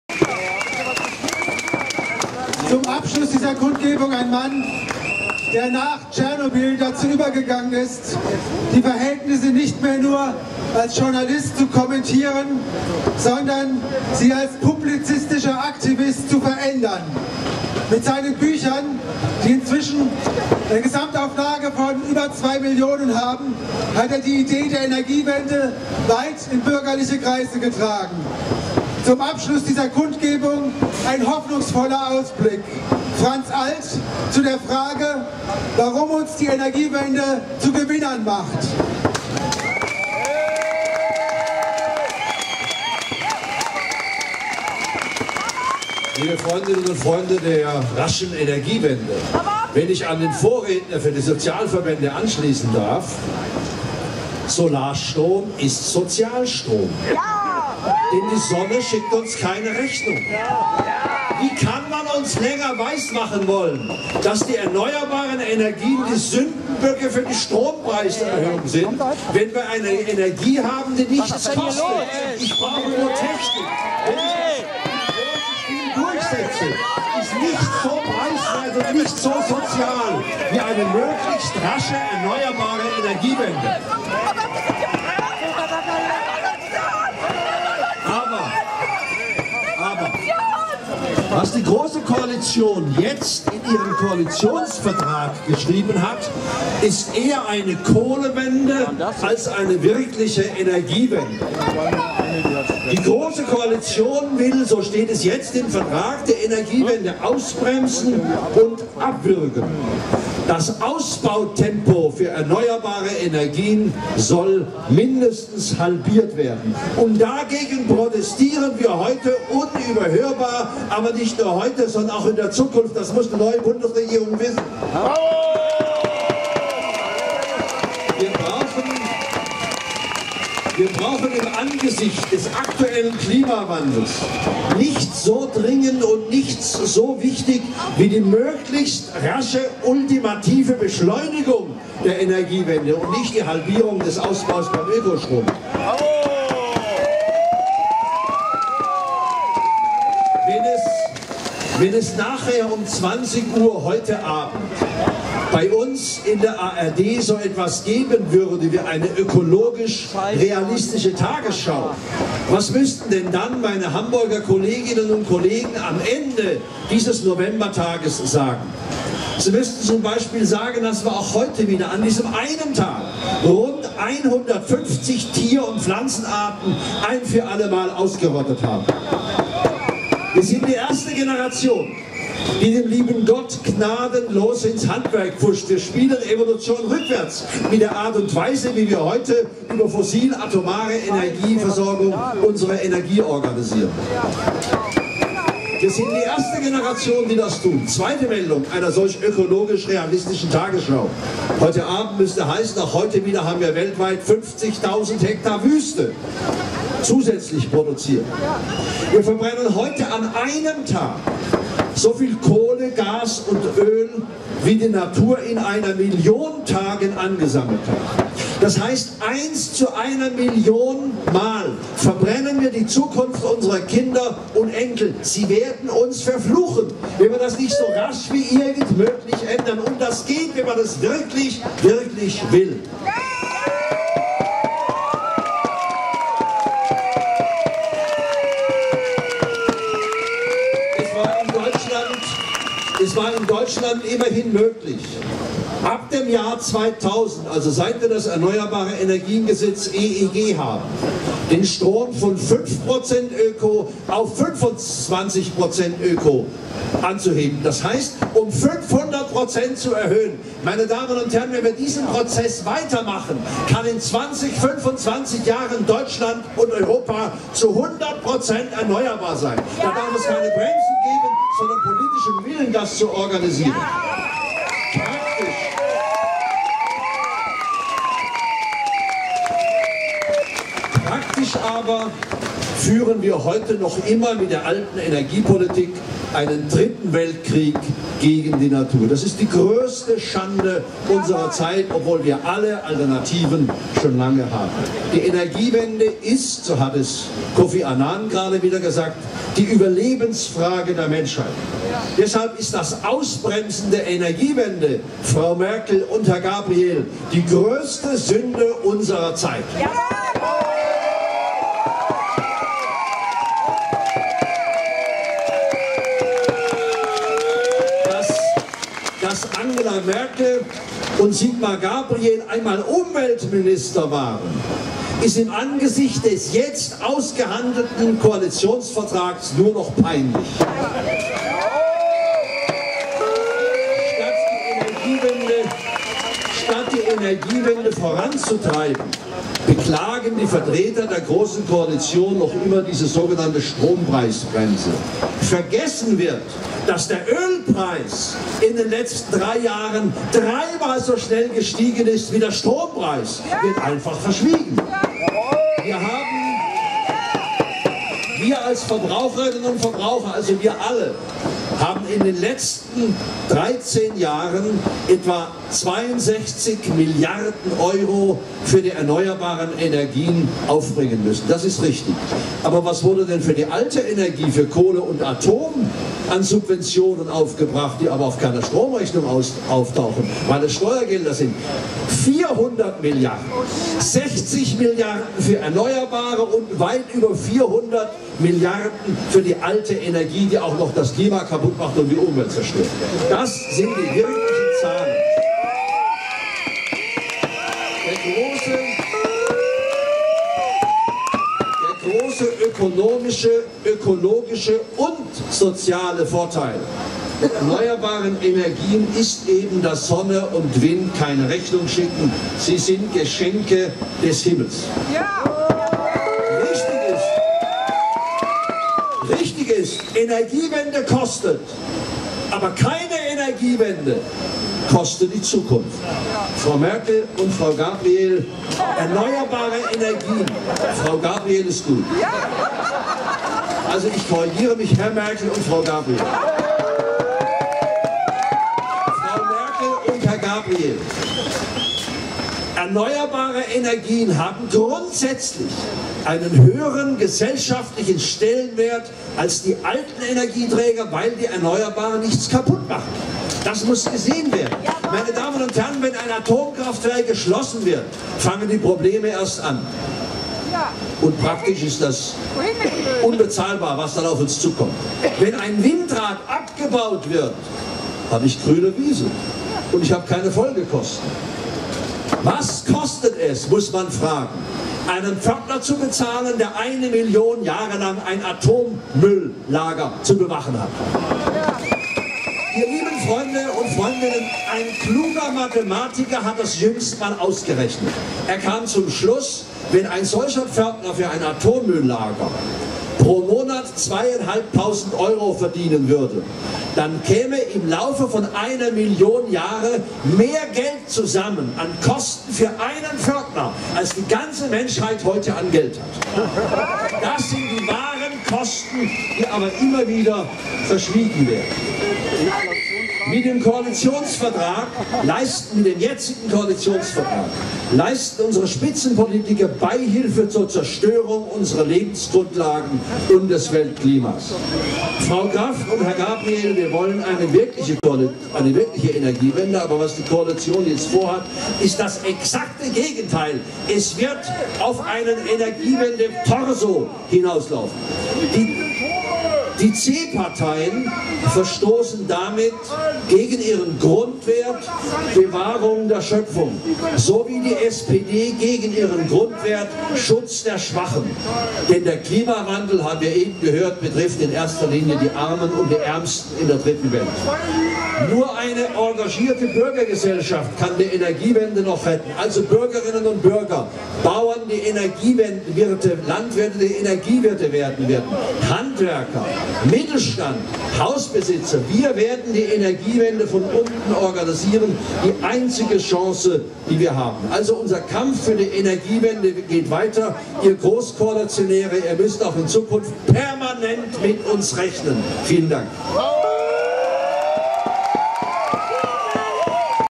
Energiewende 30112013 Berlin Part 018
Sonne und Wind statt Fracking, Kohle und Atom!
Die Energiewende ist eine riesige Chance, beim Klimaschutz ernst zu machen, Atomkraft abzuschalten, den Stromkonzernen den Stecker zu ziehen und für eine Energieversorgung in Bürgerhand zu sorgen. Genau deshalb setzen Eon, RWE, Vattenfall und Co. alles daran, dass eine neue Bundesregierung die Energiewende zurückdreht. Halten Sie dagegen! Schreiben Sie mit Tausenden anderen der neuen Regierung von Anfang an ins Stammbuch, dass Sie nicht dulden, wenn die Energiewende gebremst wird. Kommen Sie zur bundeweiten Demonstration am Samstag, 30.11., in Berlin! Schließen Sie die Umzingelung ums Bundeskanzlerinamt! Bringen Sie Ihre Energie, Ihr Transparent, Ihre Freundinnen und Freunde mit! Sagen Sie es weiter!